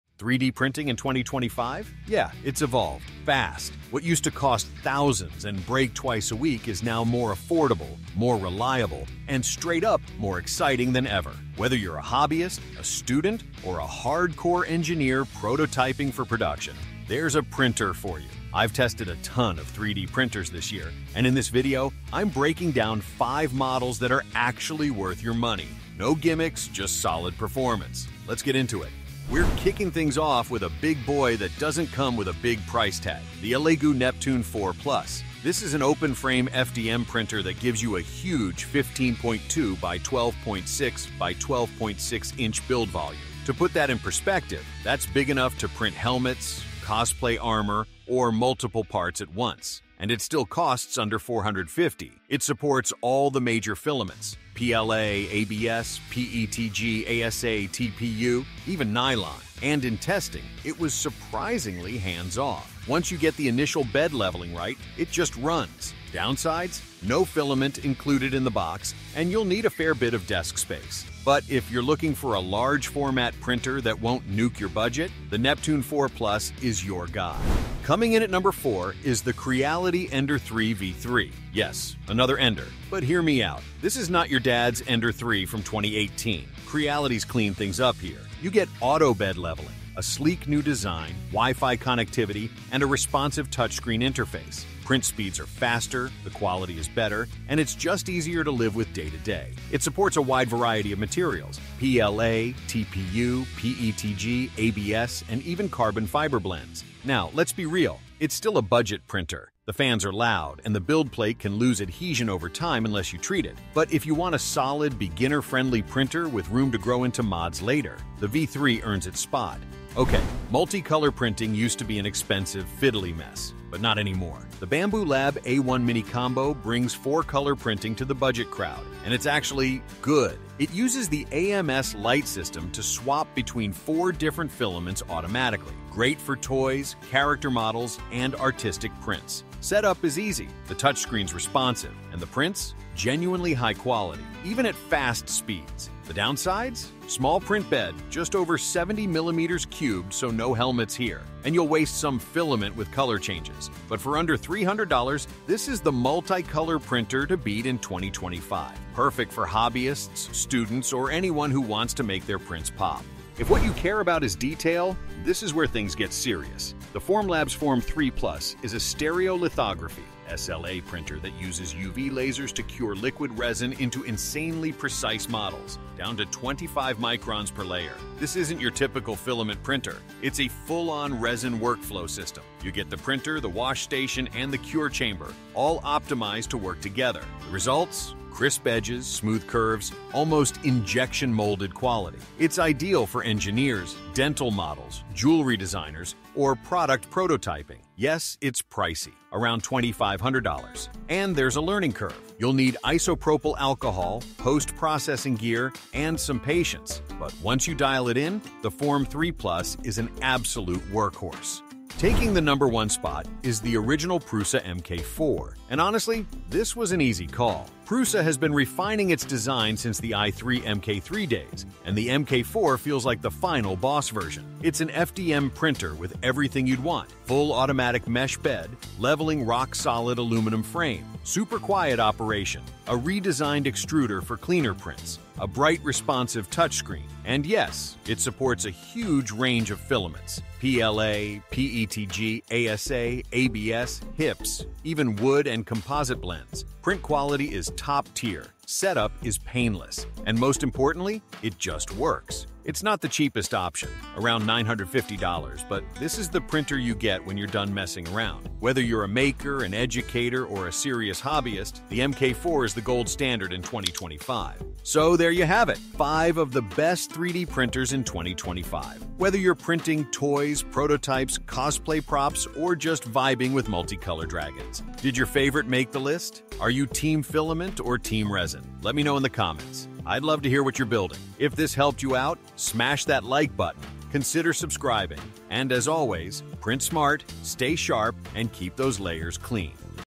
Best 3D Printers in 2025 – Don’t Buy Until You See This!
Thinking of buying a 3D printer in 2025? Hold up—this is the list you need to see. Whether you're printing cosplay armor, precision prototypes, multicolor figurines, or just getting started with your first PLA dragon, I’ve tested the top printers of the year and ranked the five that are actually worth your money.

From resin beasts to budget-friendly FDM workhorses, here’s what’s real in the world of 3D printing right now.

🛠 Covered in this video:
• Best overall printer (spoiler: it just works)
• Best entry-level machine for new makers
• Most reliable budget multicolor printer
• Pro-grade resin printing that pays off
• Large-format, high-value options under $500

🕒 Timestamps:
00:00 – Intro
00:29 – Elegoo Neptune 4 Plus
01:17 – Creality Ender-3 V3
01:59 – Bambu Lab A1 mini Combo
02:42 – Formlabs Form 3+
03:30 – Prusa MK4
04:23 – Final Thoughts

💬 What are you printing in 2025? Resin rings? Multicolor dragons? Drop your favorite printer or project in the comments.